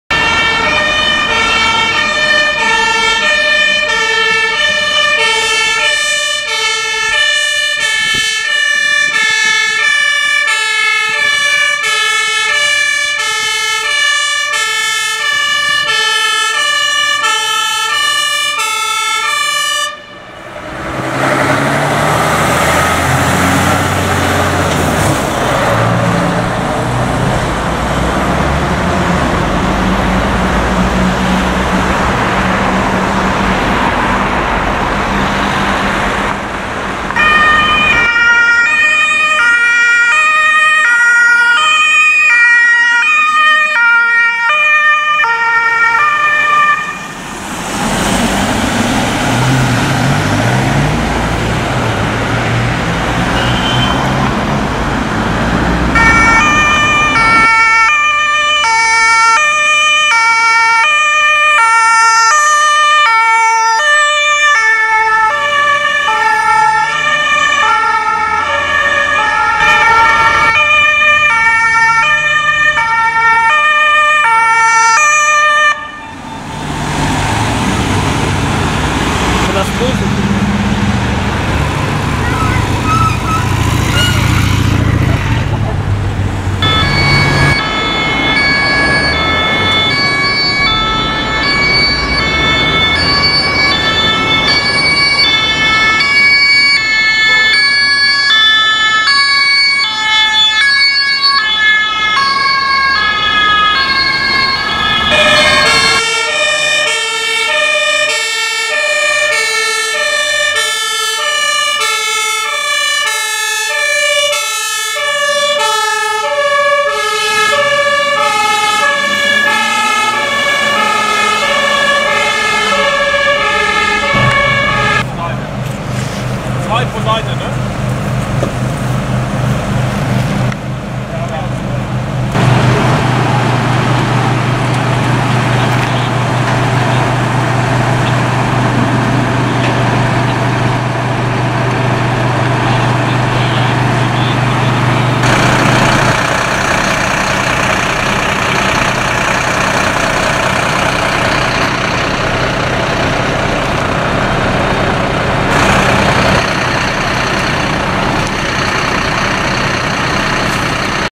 LZG (ABC-Dienst) Kreis Pinneberg Zusammenschnitt Einsatzfahrten+Übungsort
Hier zu sehen mehrere verschiedene Einsatzfahrten des ABC Dienstes Kreis Pinnberg zu verschiedenen Einsätzen.
Außerdem zu sehen der Übungsort von der LZG Übung im letzten Jahr.